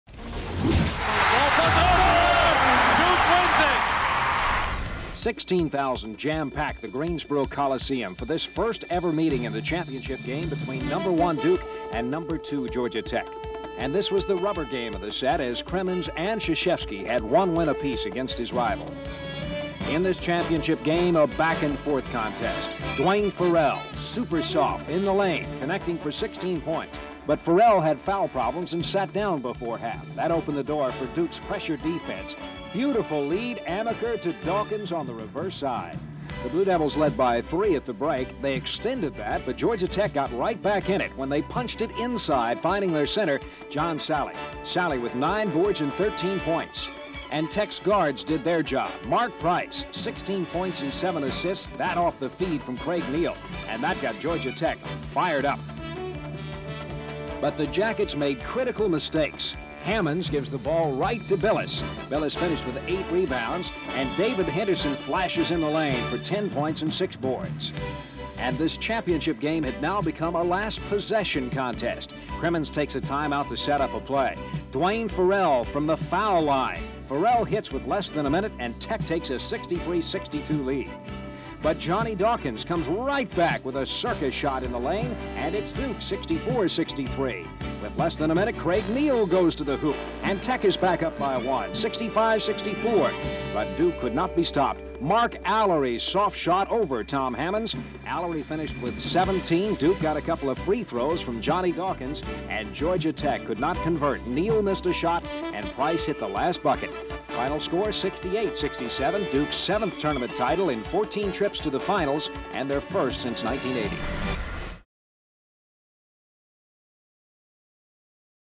Duke's First ACC Tournament Championship Under Coach K | Coach 1K Moments
As we count down to Mike Krzyzewski's 1,000th career victory, we take you back through some of Coach K's most memorable moments as Duke head coach.  In this Coach 1K Moment, Duke overcomes a late Georgia Tech push to capture the 1986 ACC Tournament Championship. This championship was not only the first since 1980 for Duke, but also the first of thirteen titles during Coach K's tenure.